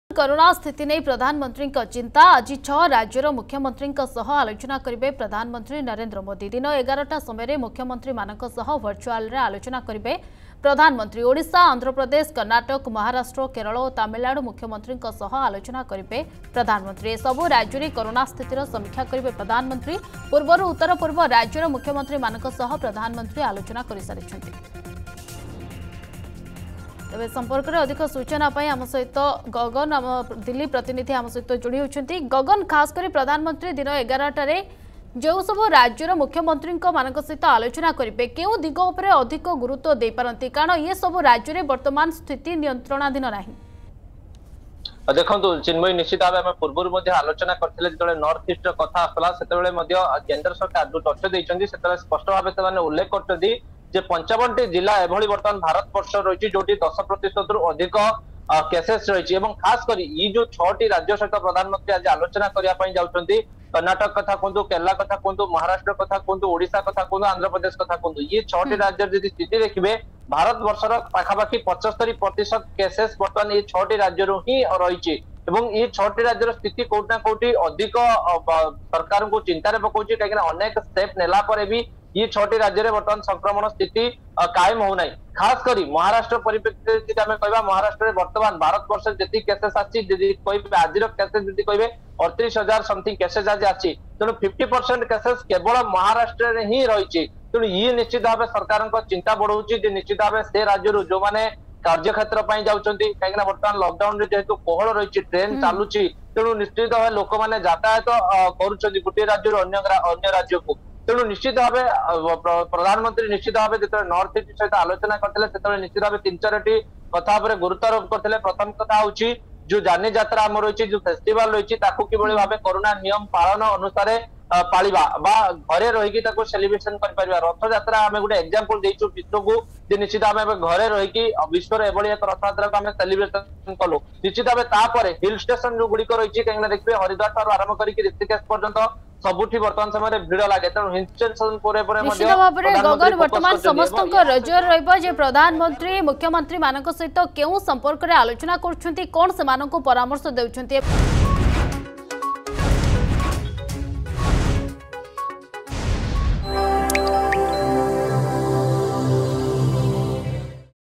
କରୋନା ସ୍ଥିତି ନେଇ ପ୍ରଧାନମନ୍ତ୍ରୀଙ୍କ ଚିନ୍ତା
କରୋନା ସ୍ଥିତି ନେଇ ପ୍ରଧାନମନ୍ତ୍ରୀଙ୍କ ଚିନ୍ତା । ଆଜି ୬ ରାଜ୍ୟର ମୁଖ୍ୟମନ୍ତ୍ରୀଙ୍କ ସହ ଆଲୋଚନା କରିବେ ପ୍ରଧାନମନ୍ତ୍ରୀ ନରେନ୍ଦ୍ର ମୋଦି ।